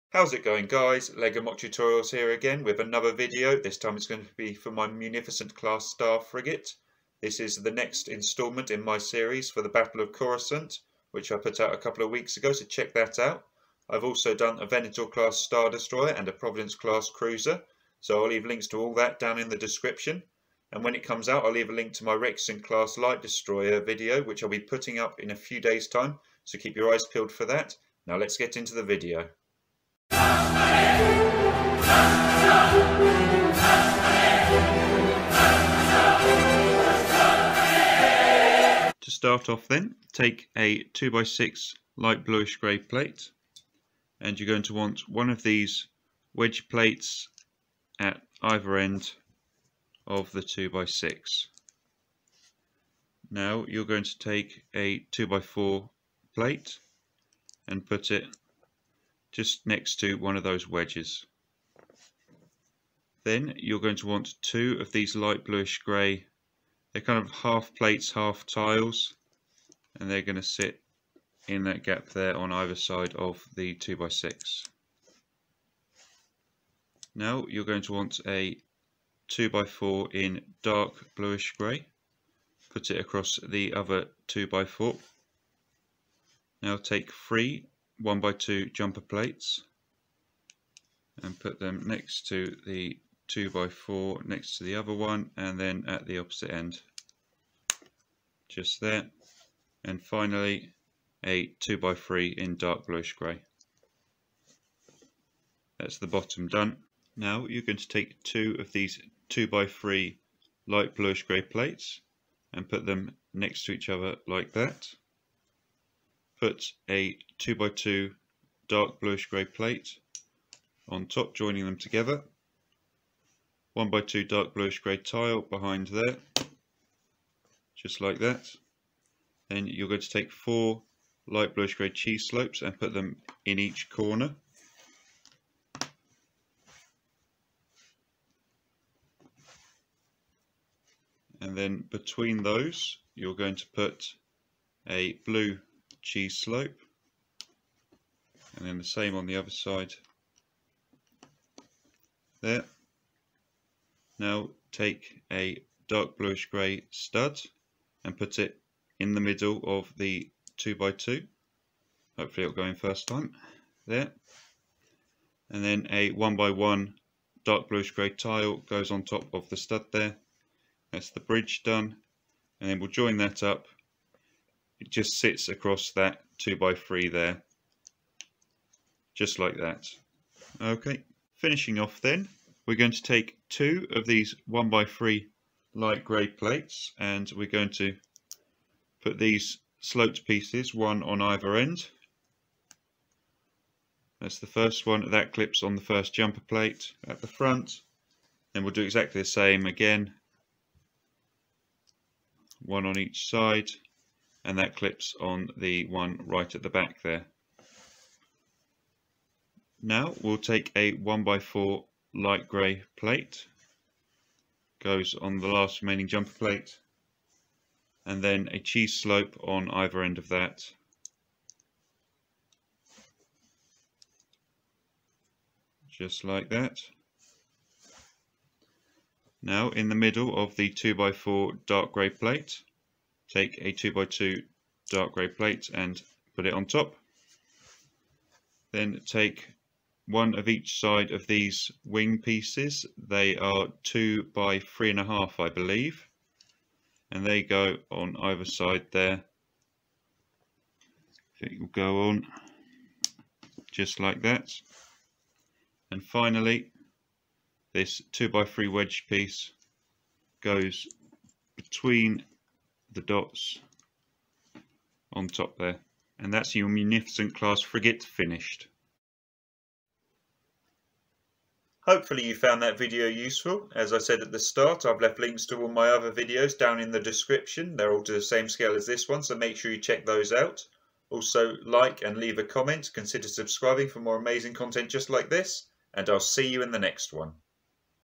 Lego Munificent Class Star Frigate Build Tutorial (Banking Clan Comms Ship) Star Wars Revenge Sith
This is my custom design microscale Munificent Class Star Frigate.